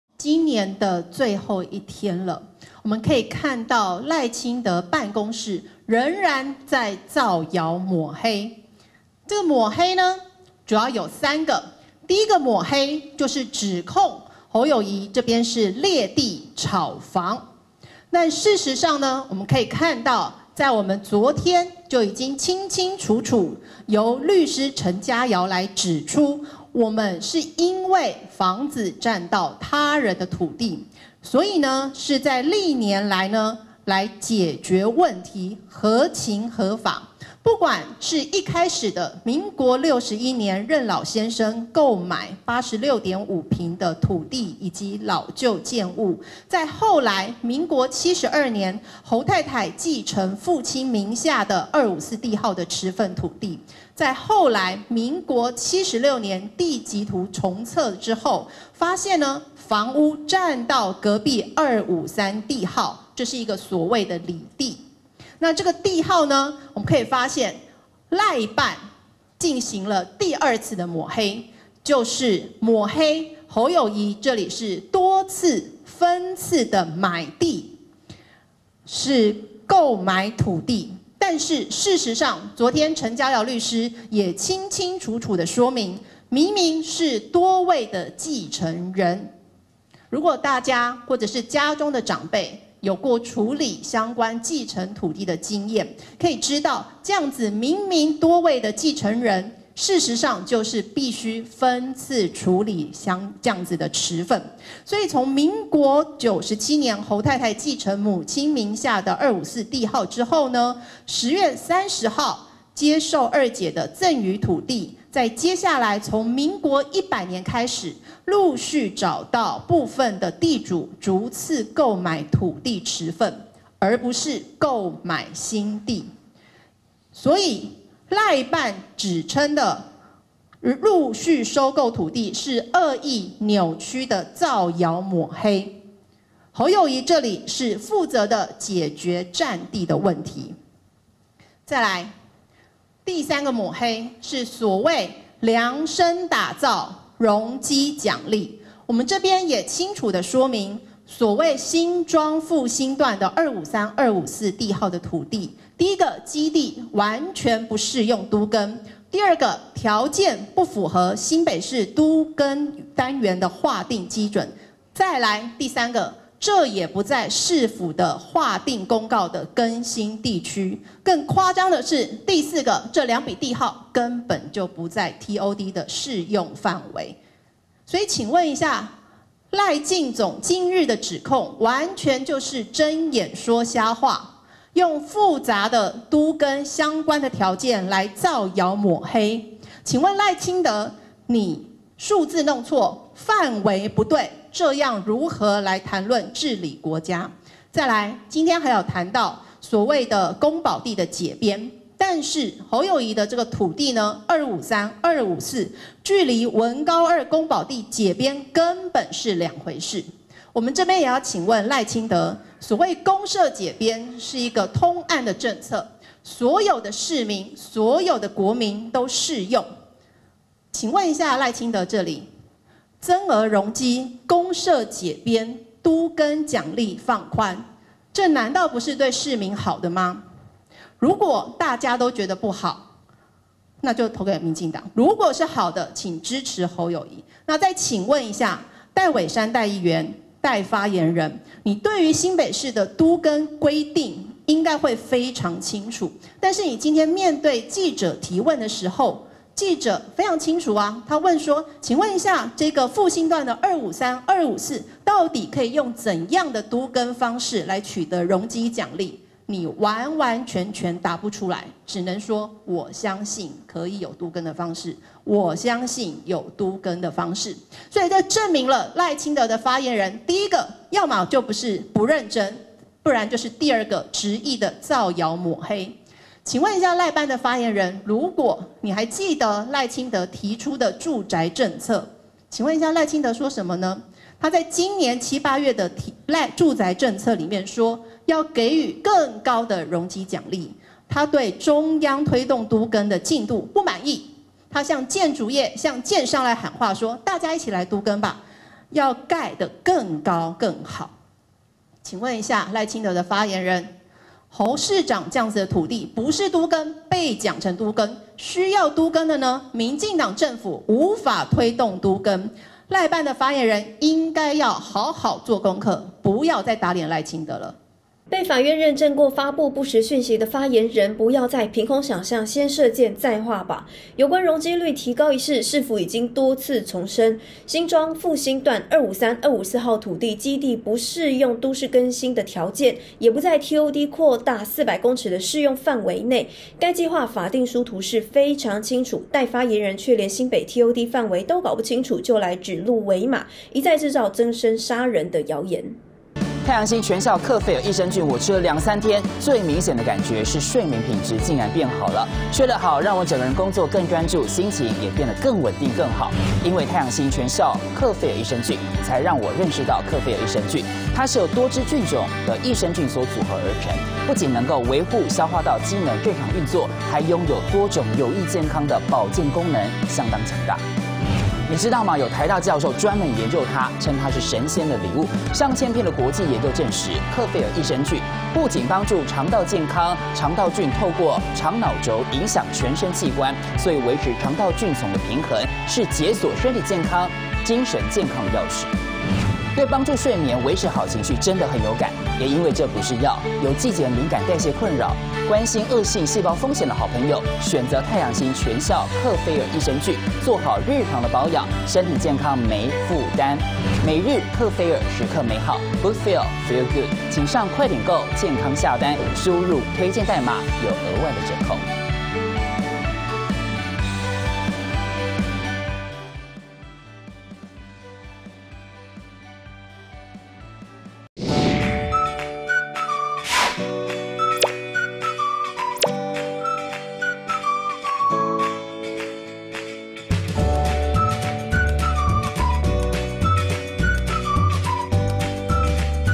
重砲反擊新莊獵地揪賴陣營三大抹黑老哏! 江怡臻怒噴戴瑋姍烏龍爆料酸:不是不認真就是造謠
責任小編:老大不小編/肉肉編